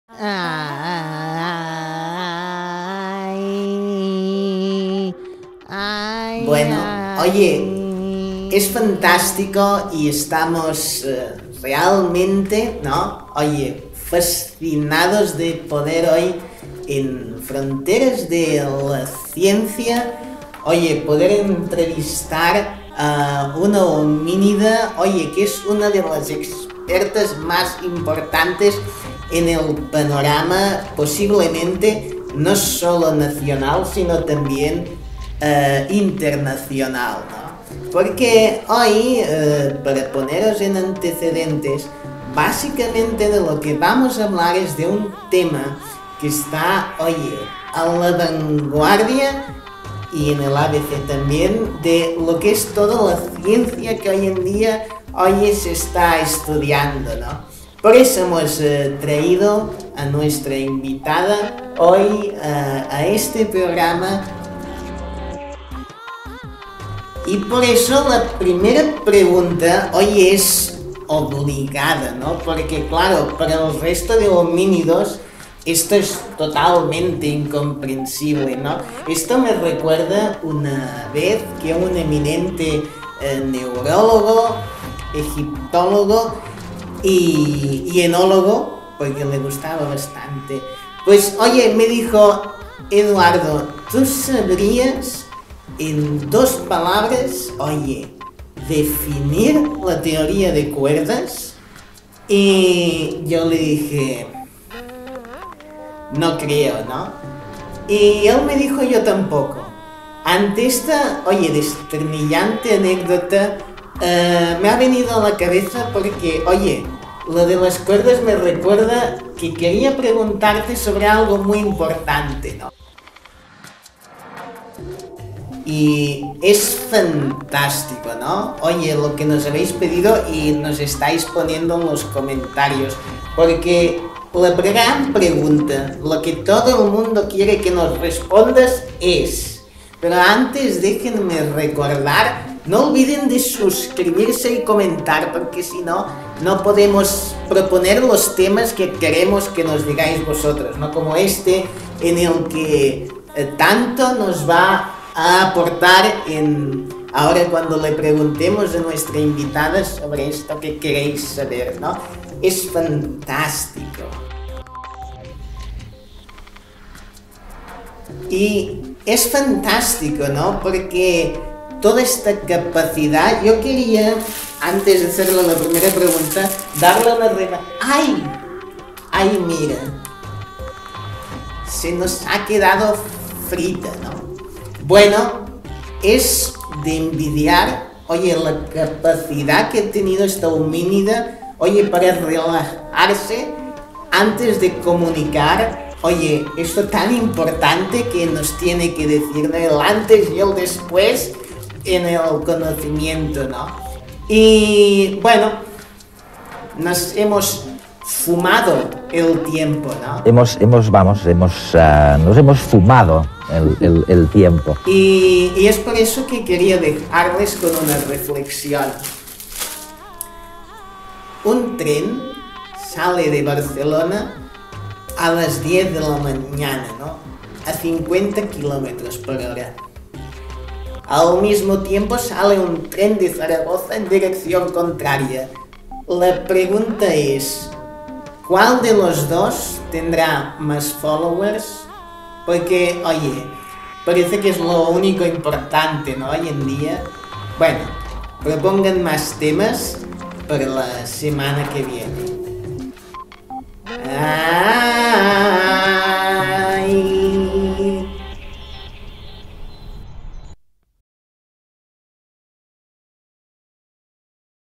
Fronteras de la Ciencia (Parodia Redes)
Hoy un personaje clásico de la radio de David Cervelló, el Punset que tantas alegrías y risas nos ha dado. ¿Logrará hacer una pregunta a la homínida?

@davidcervello (twitter)